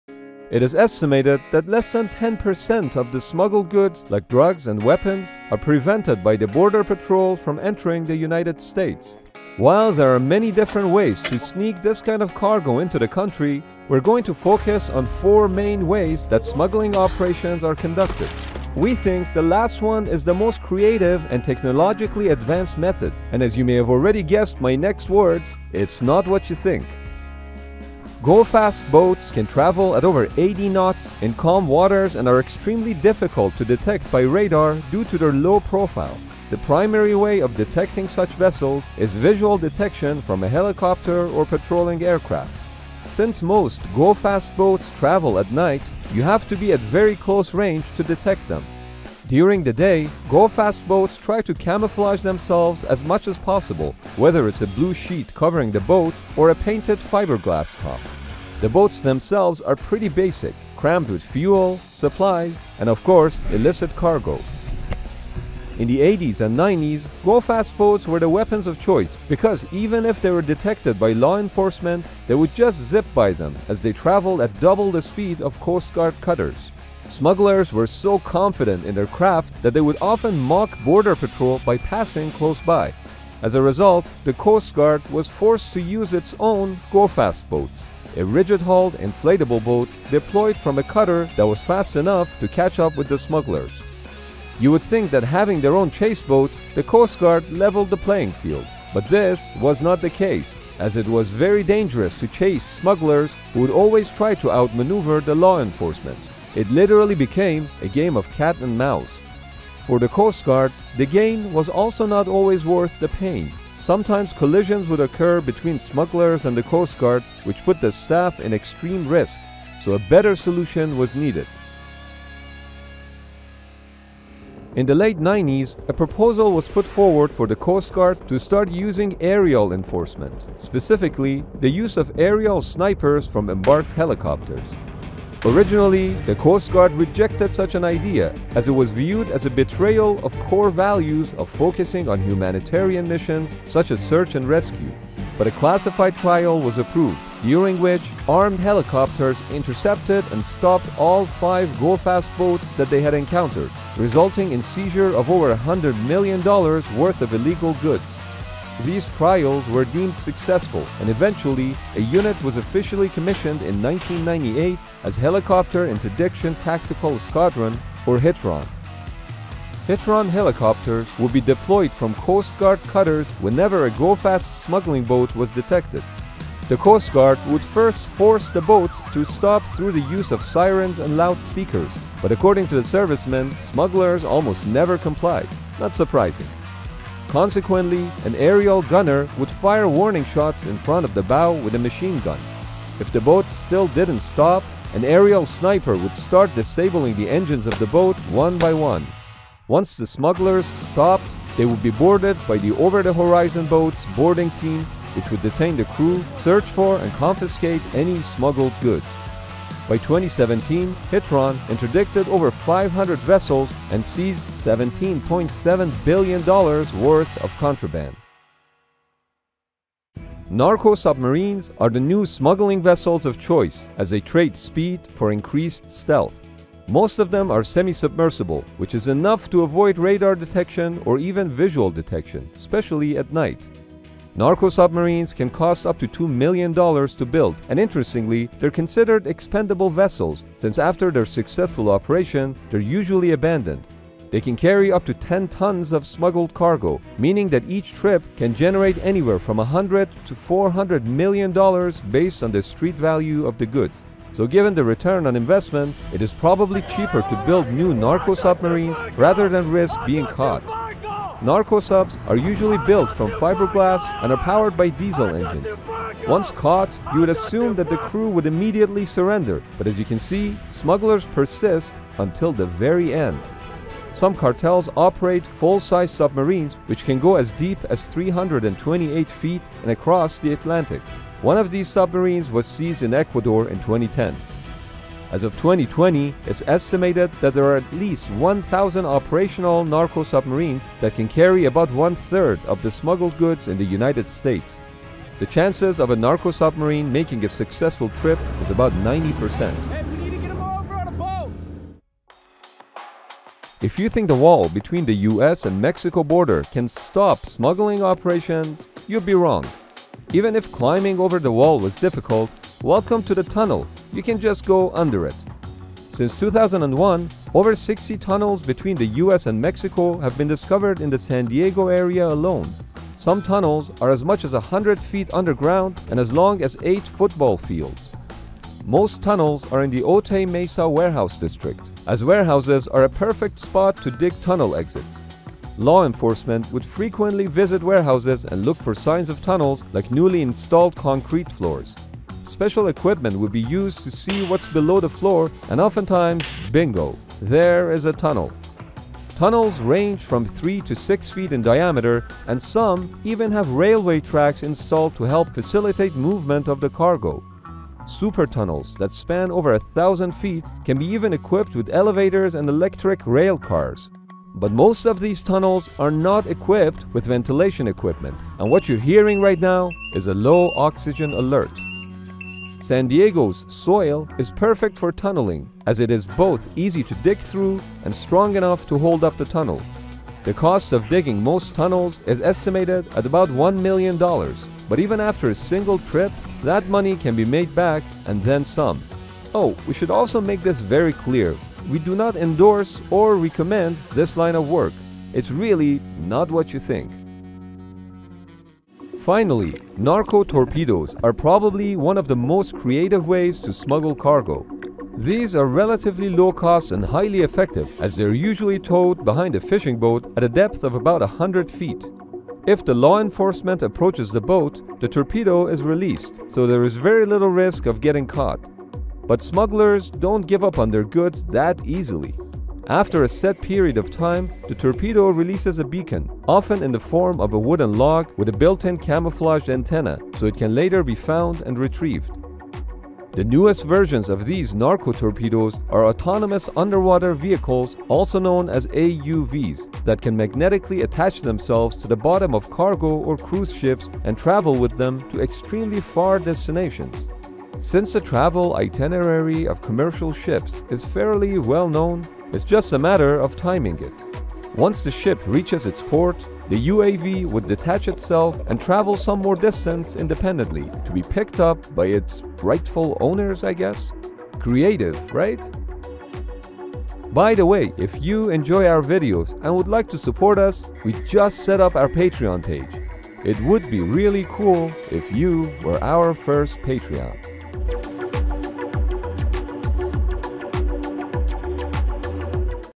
4 Ways Smugglers Operate
You may not have looked for it, but here it is. Four ways that smugglers bring goods into the United States, and some of them are NotWhatYouThink!